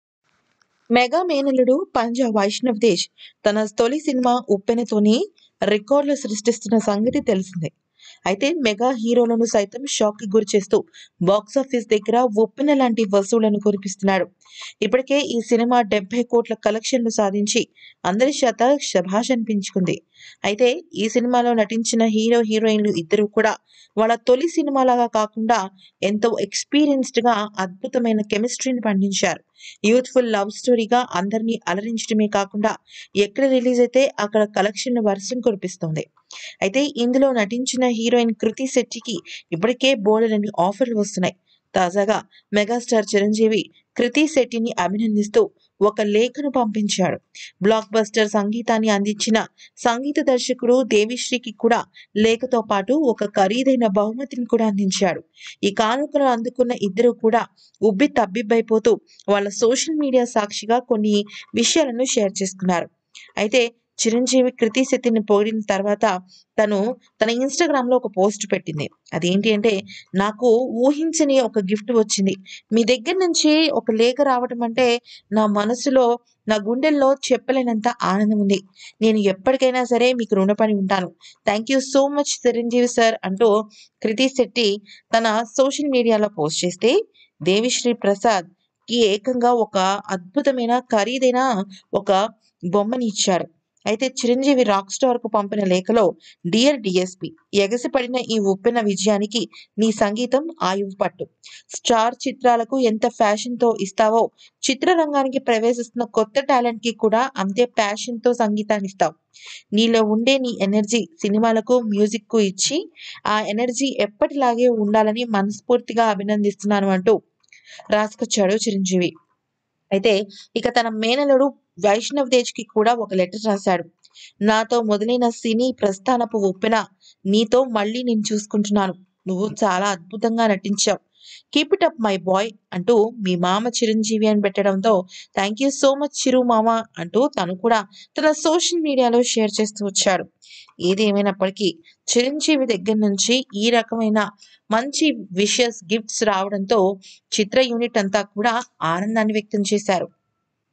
ఉప్పెన సినిమా హీరోయిన్ కృతి శెట్టికి ఊహించని గిఫ్ట్ పంపిన చిరు || Chiranjeevi gifts krithi setty
Uppen movie || Panja vaishnav teja || Megastar chiranjeevi || devisriprasad